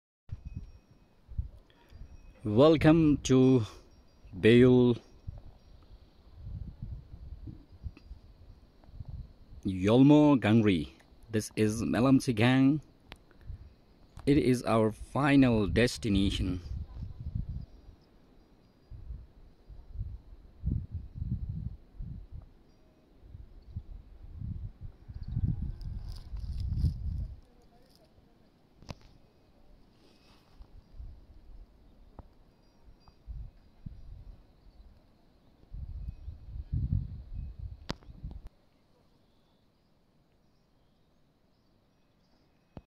Melamchigyang Village||Helambu Trek Nepal
helambutrek Helambu is one of Beyul (Hidden Valleys) in Nepal. This place blessed many lamas, and dakinies including Gurupadmasambhawa.